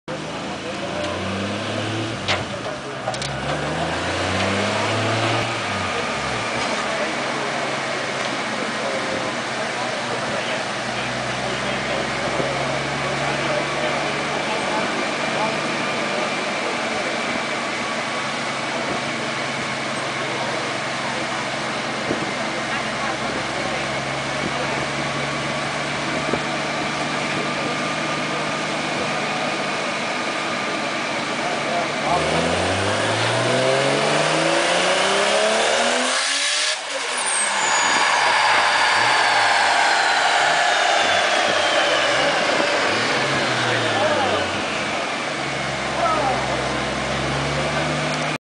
Jerry's turbo Supra Dyno
car made 380 whp @ 11 psi on a t61. only went to 6k rpms.